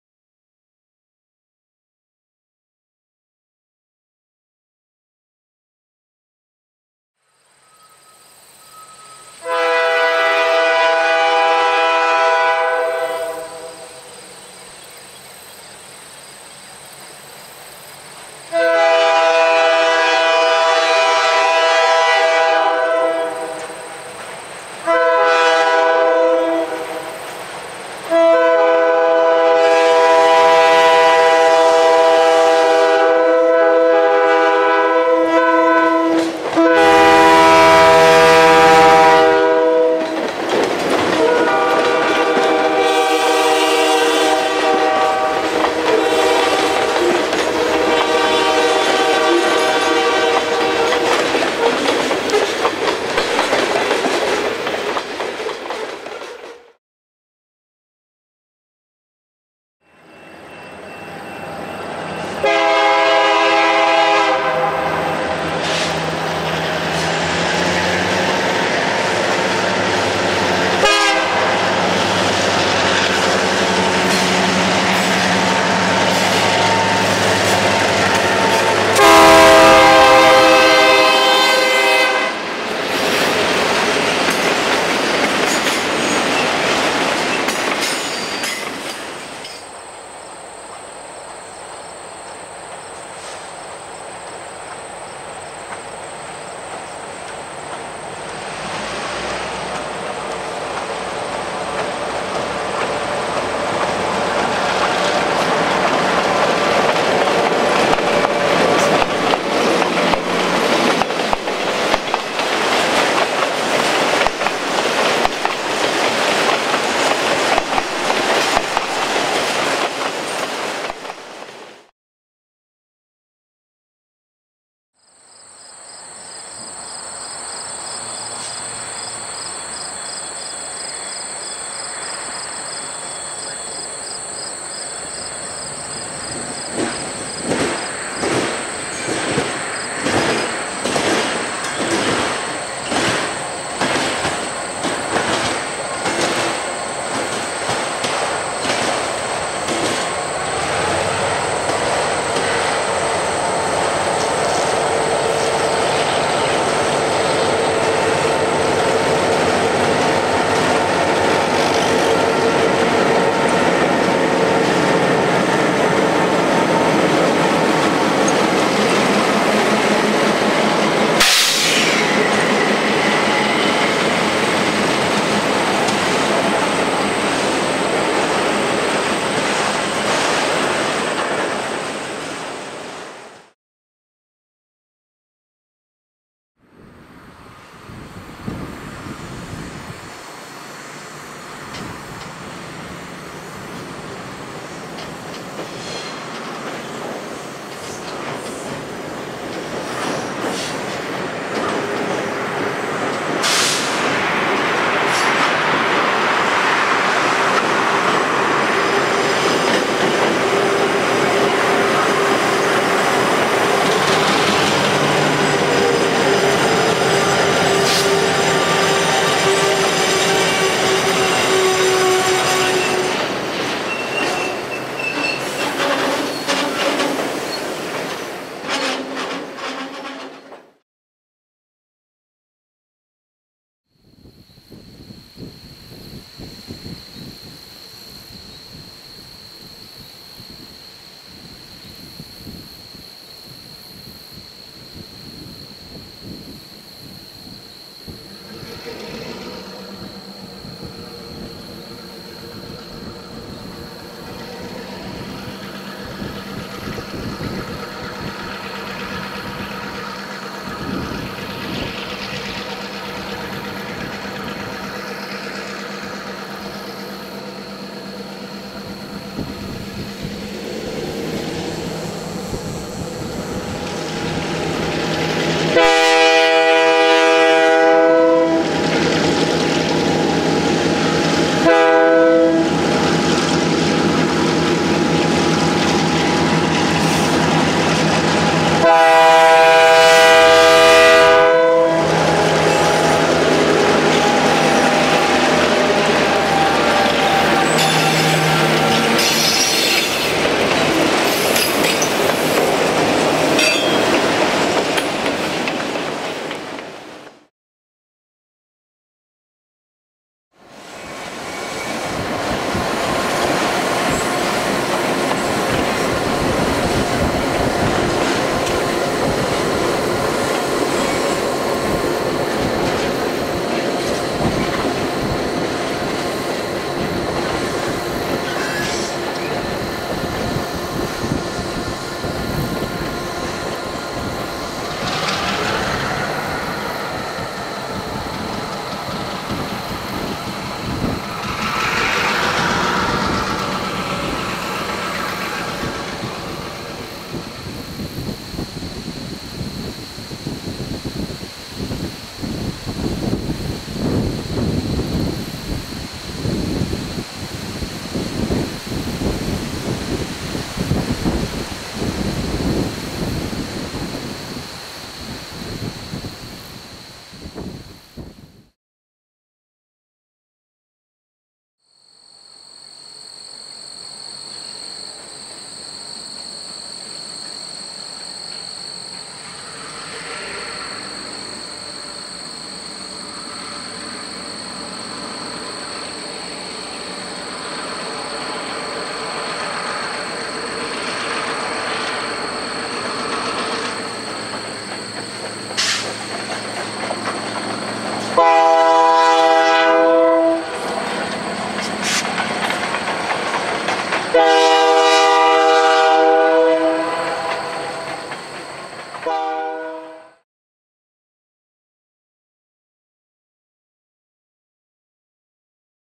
Ontario Southland Railway - Woodstock Job
After running light from their shop in Salford, Ontario, GP7 383 and FP9A 6508 are first seen heading west to Putnam with empty hoppers.

We then pick them up eastbound at Beachville, after lifting Cami Automotive in Ingersoll.  It's a straight shot to Woodstock where they'll yard the train and pick up empties to return to the auto plant.

Departing Woodstock, it's another straight shot, this time to Ingesoll.  We next see them shoving the empties into Cami before departing light for Salford.

James Adeney
Toronto, Canada